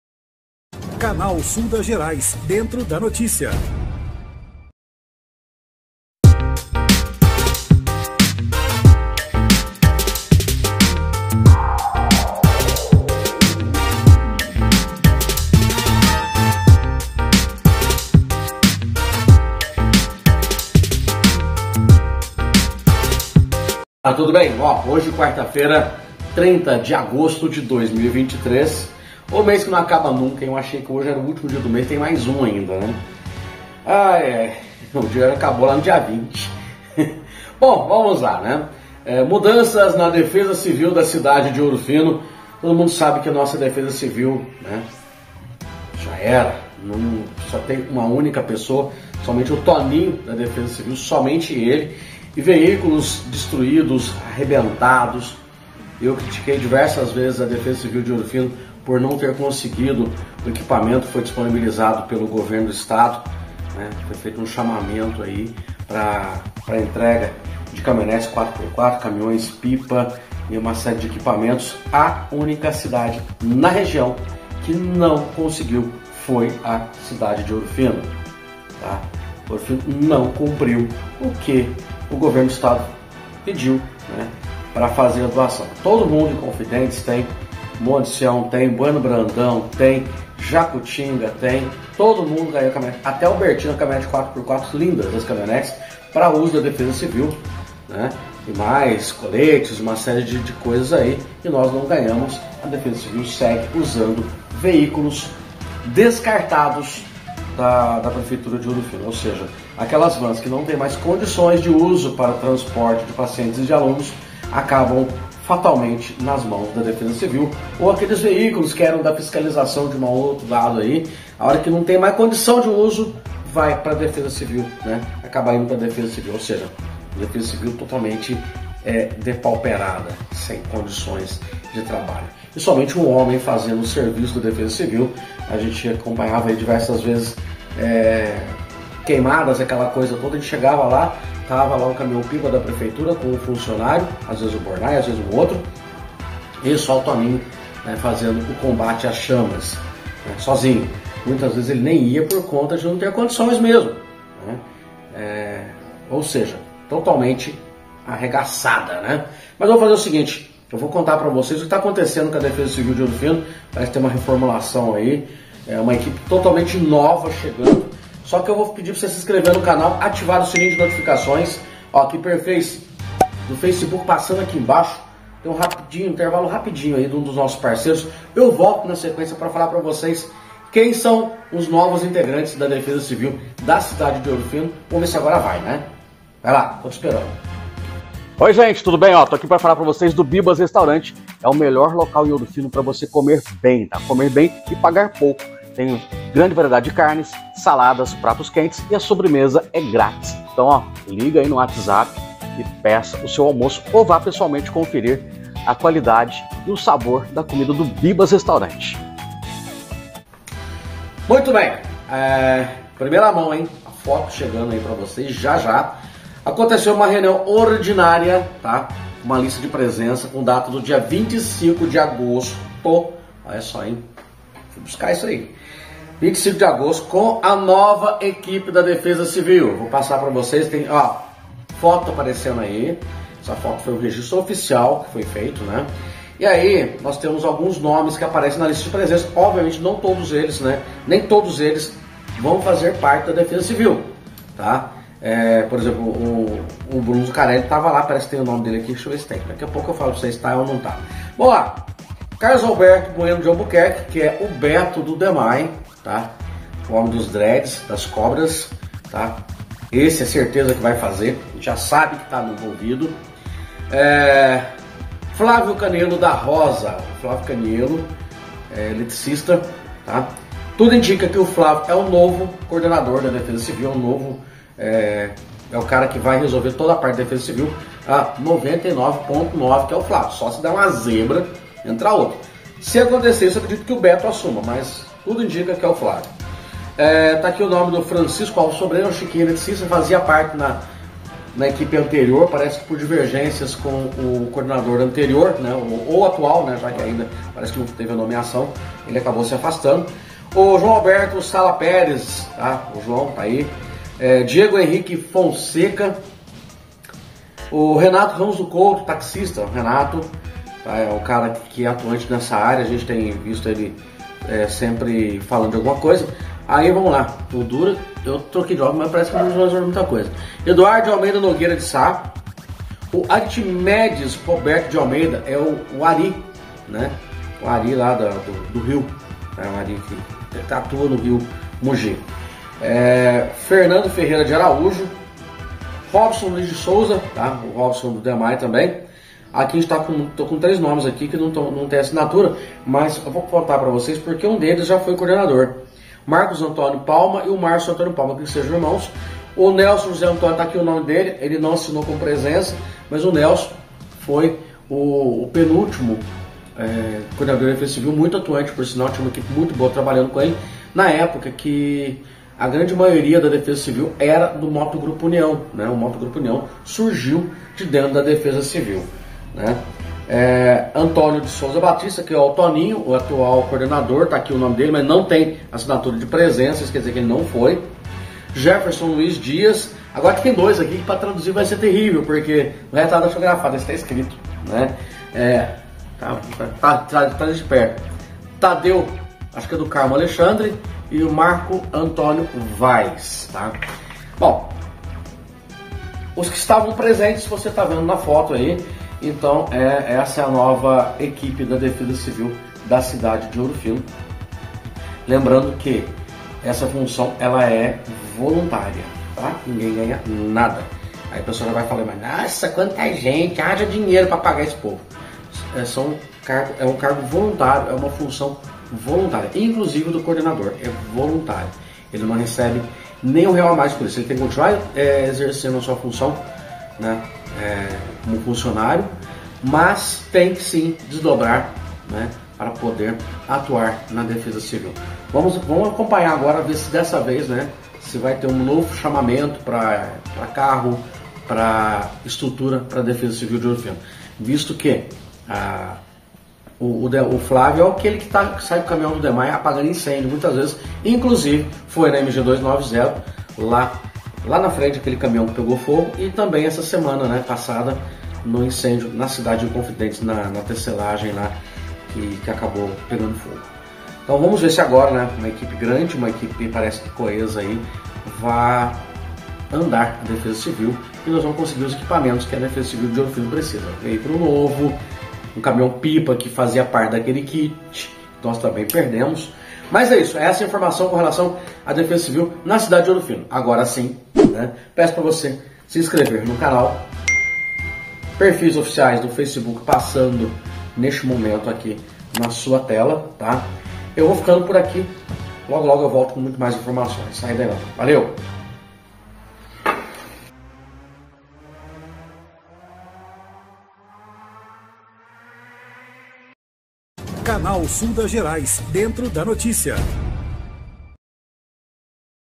DEPOIS DE PASSAR POR VÁRIOS PROBLEMAS, DEFESA CIVIL DE OURO FINO TEM NOVA EQUIPE
Nova equipe da Defesa Civil de Ouro Fino MG, Flávio Caniello será o novo Coordenador do órgão no município.